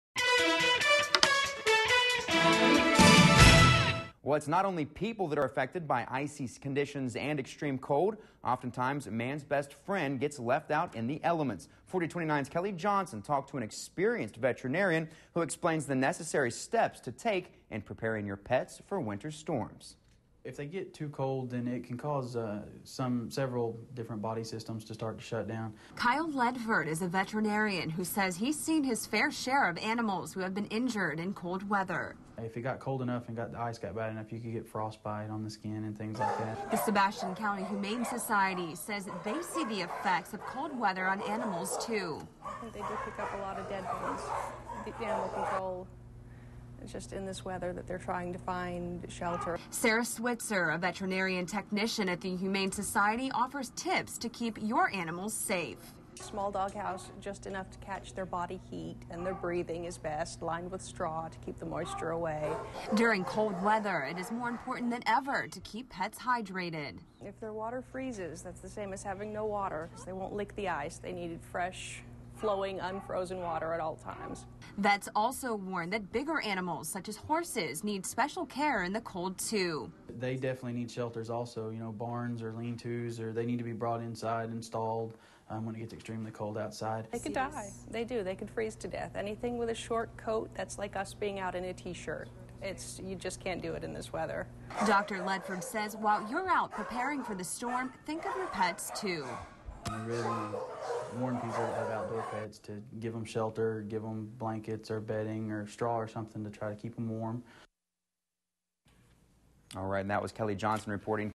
Veterinarians: Cold Weather Poses Threats To Pets
Veterinarians and the Sebastian County Humane Society explain the necessary steps to take in preparing your pets for winter storms.